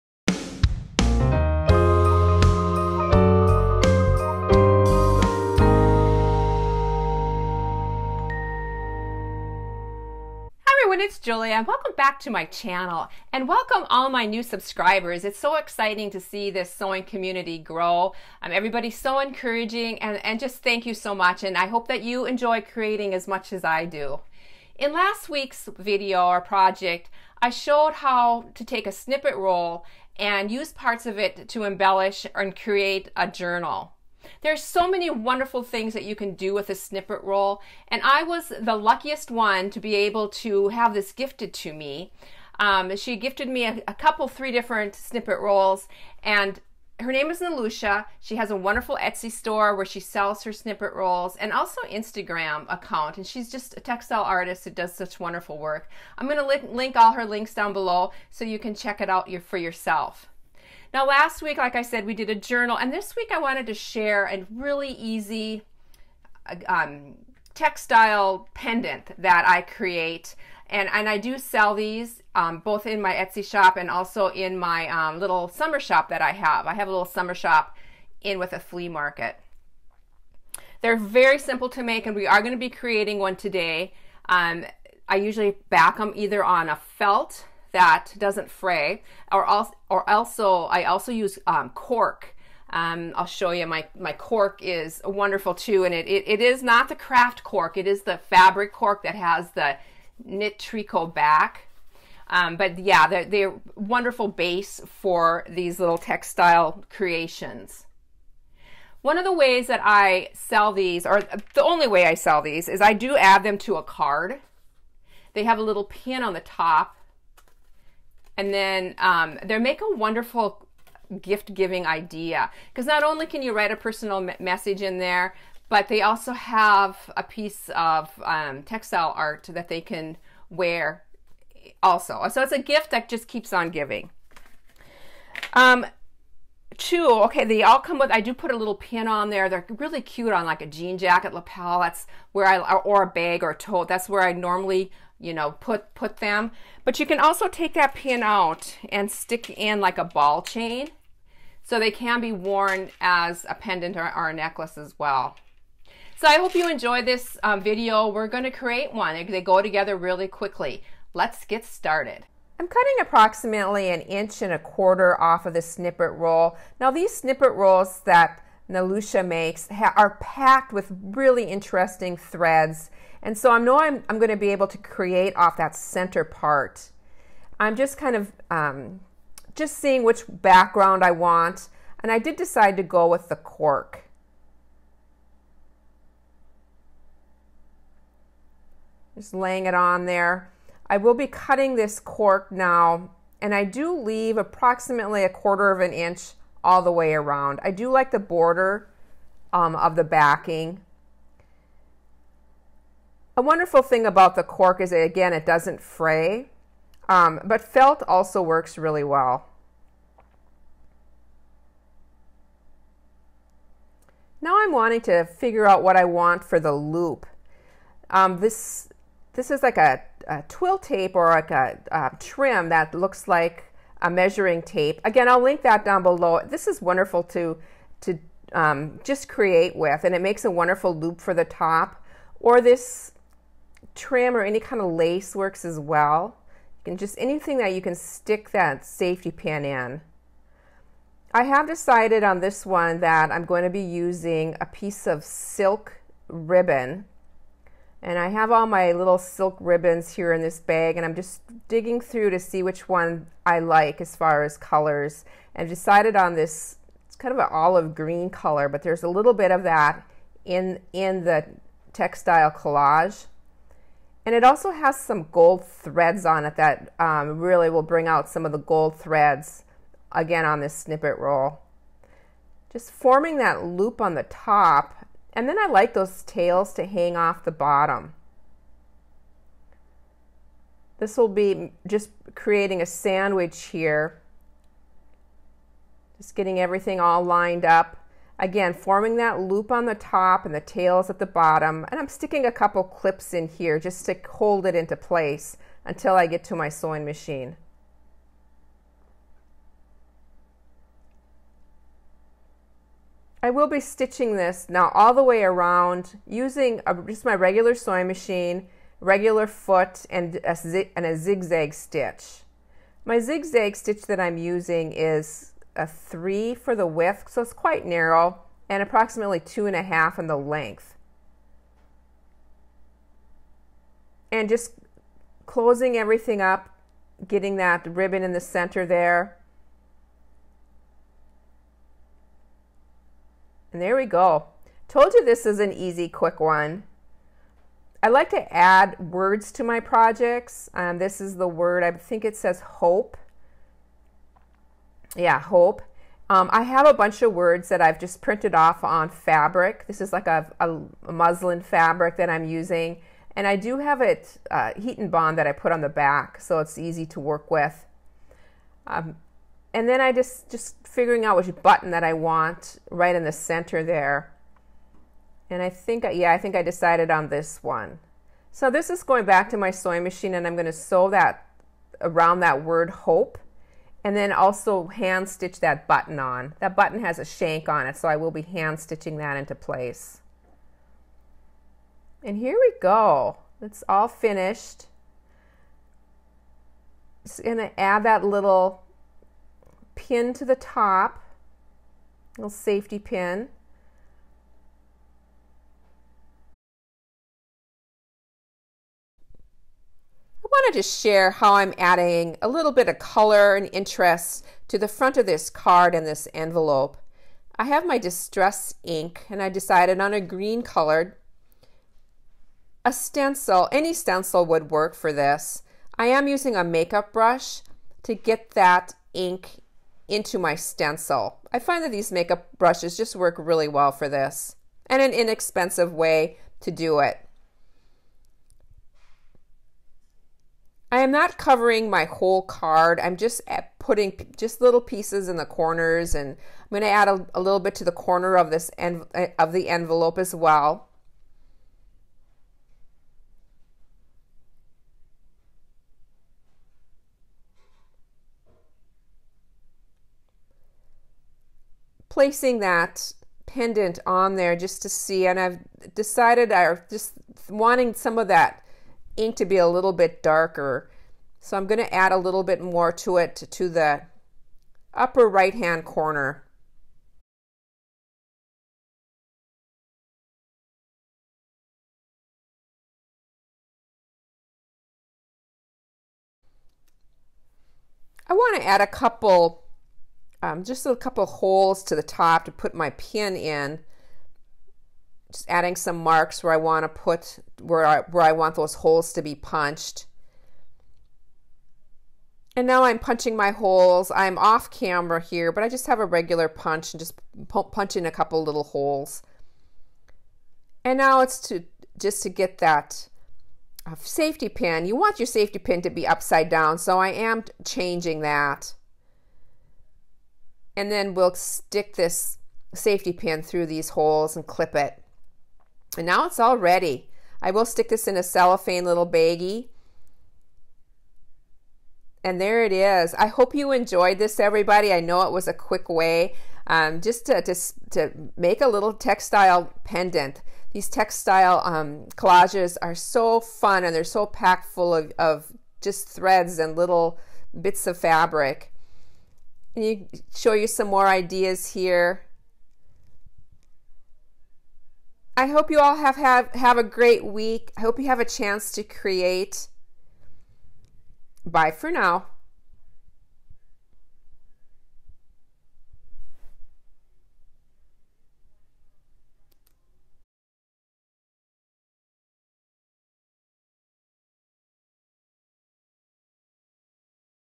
Jewelry Made From a Fabric Snippet Roll, Create with Me
Create some colorful, whimsical fabric pendants from a snippet roll.  This is such a easy and quick projects!

Follow Nelusha's creative journey on her Instagram: @kitschychic

SUPPLIES
Cork fabric

Measuring tape trim

Wonder Clips

Sewing Machine (Memory Craft 8200)

Distress Ink (Green)

Make-up Brushes ( used to apply ink)

Card Base with envelopes

A few of my favorite things

Etsy Shop